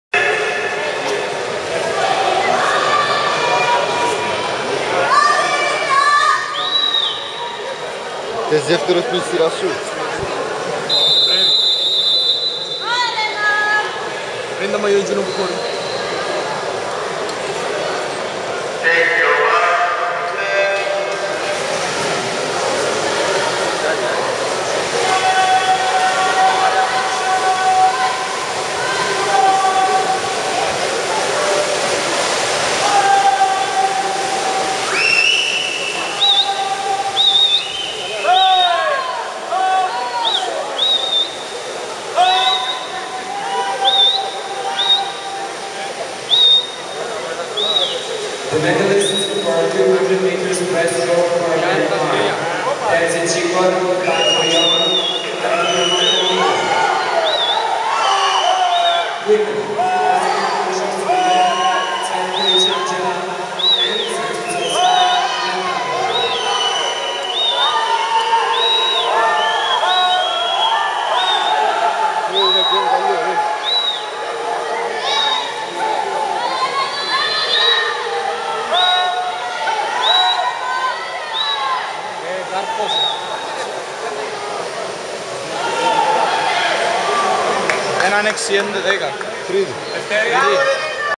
Hatzikyriakou Elena swimming the 100 Butterfly at the Balkan Junior Championship 2012
Balkan swimming champioship Krusevac 2012,
Elena is in lane 2 and Finished 3rd!!!